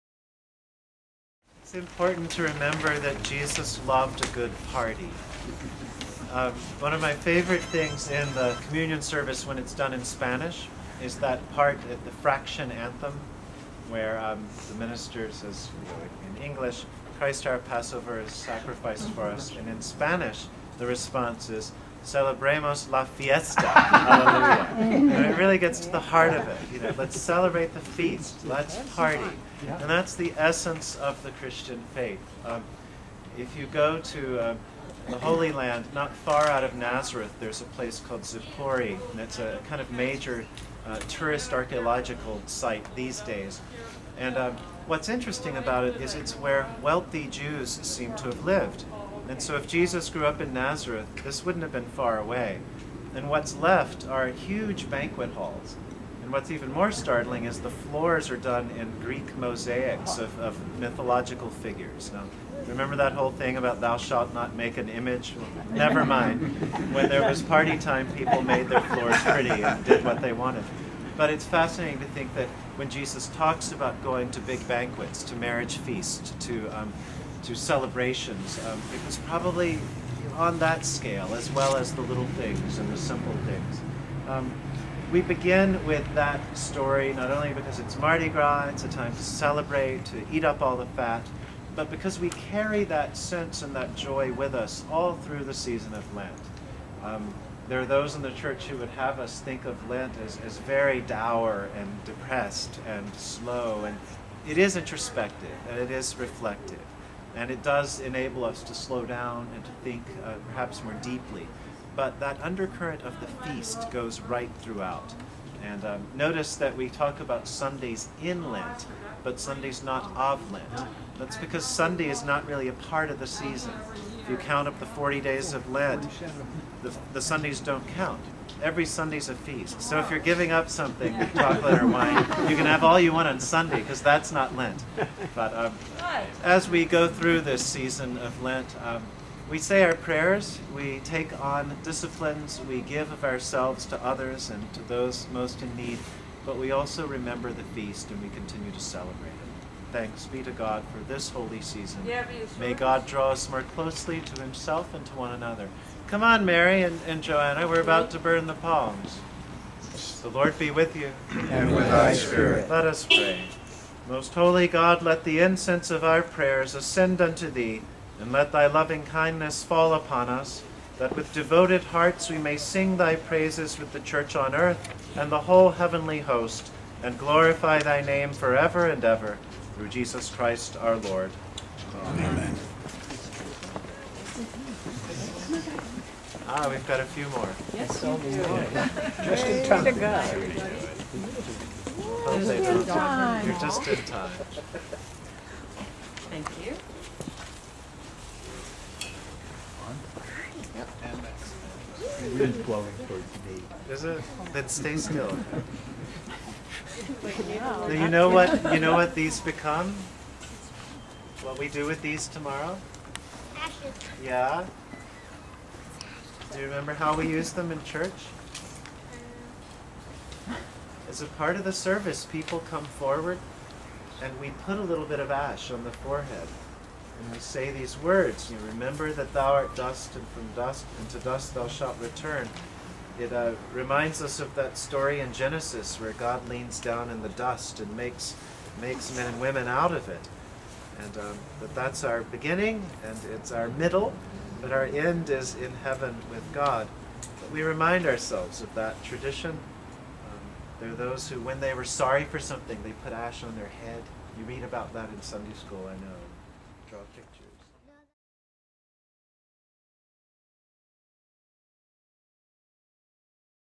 Shrove Tuesday 2009 All Souls-DC
Shrove Tuesday 2009 --- The Rev. John Beddingfield, Rector of All Souls Memorial Episcopal Church in Washington, DC directs the burning of the palms with his trademark wit and reverence.